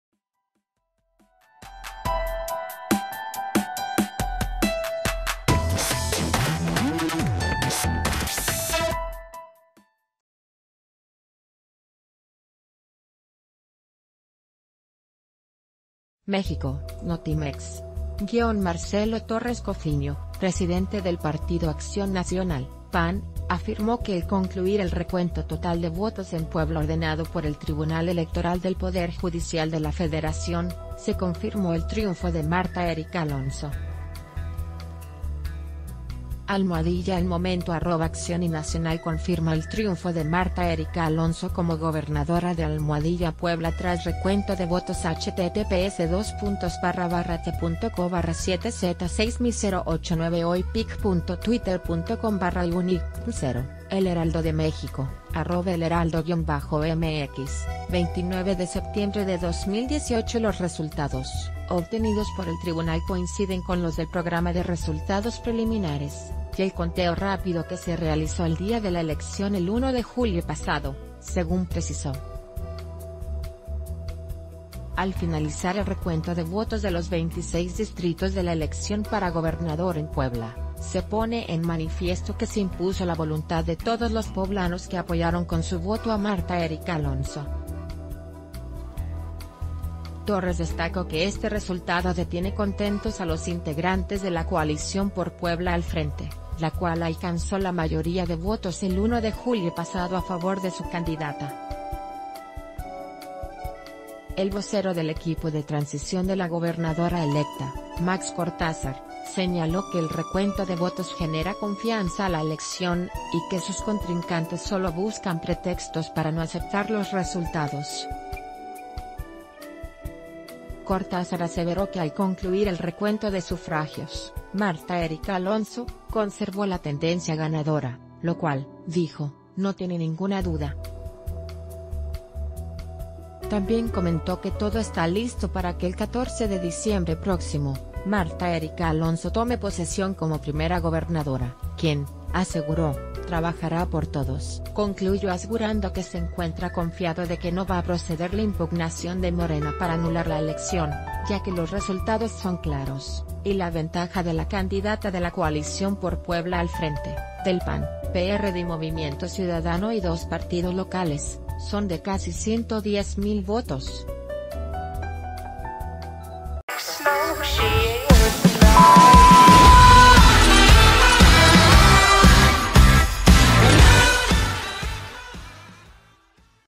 PAN ratifica triunfo de Martha Érika Alonso en Puebla - El Diario de Yucatán
MÉXICO (Notimex).- Marcelo Torres Cofiño, presidente del Partido Acción Nacional (PAN), afirmó que el concluir el recuento total de votos en Puebla ordenado por el Tribunal Electoral del Poder Judicial de la Federación, se confirmó el triunfo de Martha Éricka Alonso. AlMomento @AccionNacional confi...